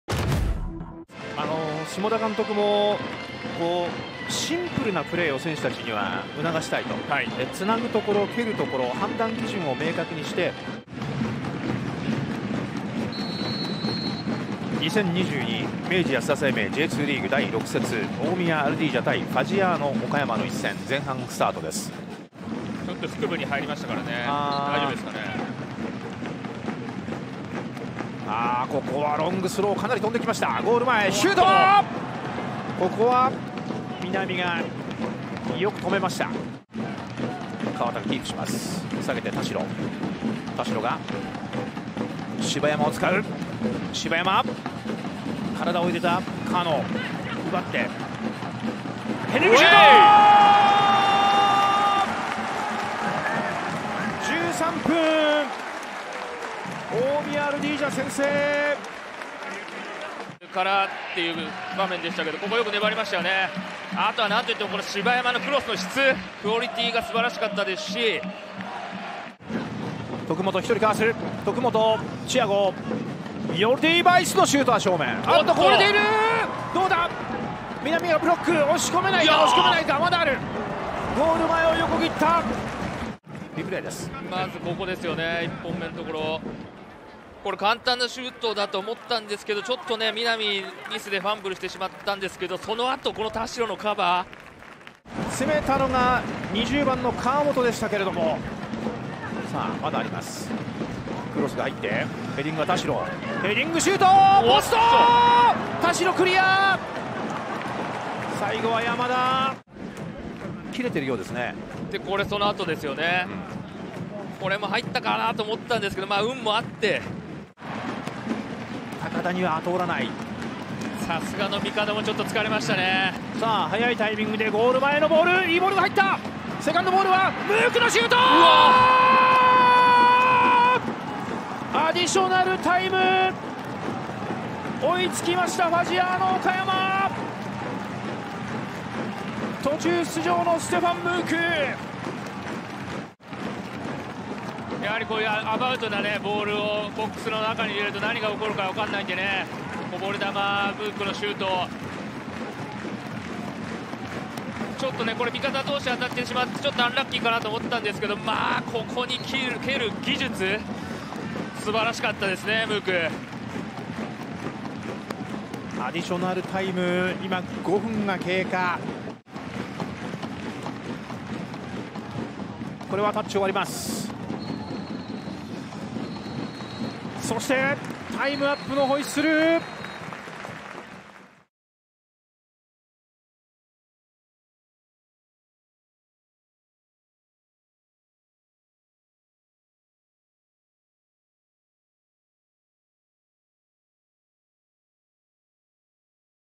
【公式】ハイライト：大宮アルディージャvsファジアーノ岡山 明治安田生命Ｊ２リーグ 第6節 2022/3/26
2022年3月26日（土）に行われた明治安田生命Ｊ２リーグ 第6節 大宮vs岡山のハイライト動画です。